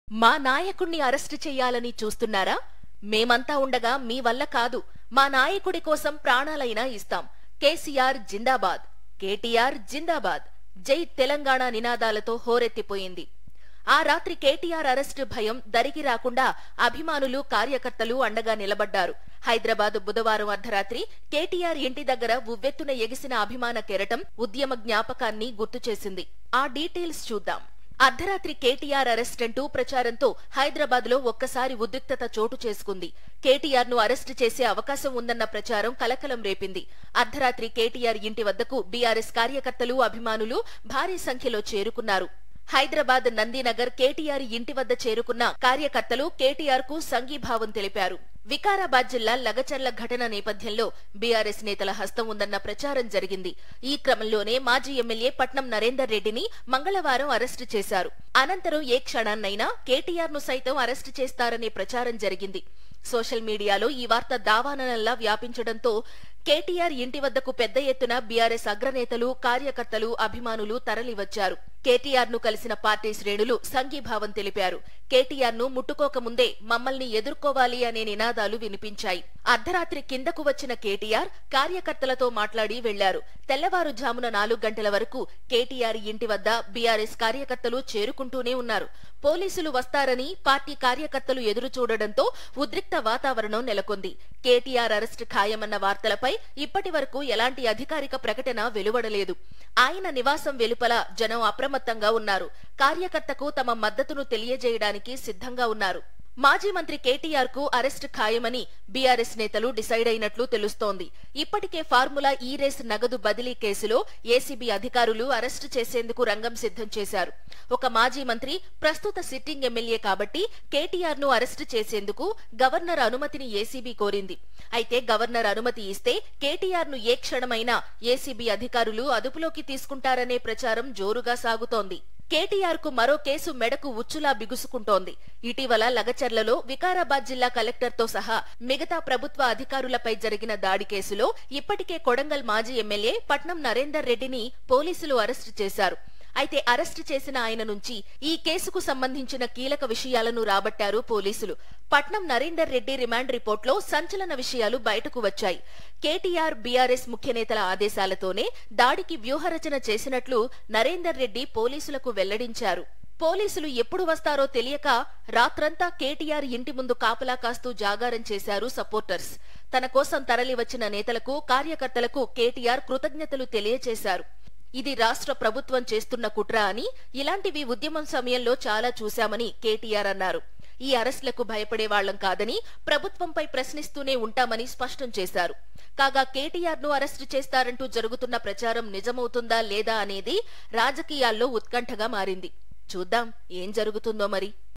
ఏ క్షణాన్నయినా కేటీఆర్ అరెస్ట్..! ktr revanthreddy | B1NEWS TELUGU
ఫేస్‌బుక్, ఇన్‌స్టాగ్రామ్, ట్విటర్‌లలో B1 News తెలుగును ఫాలో అవ్వండి.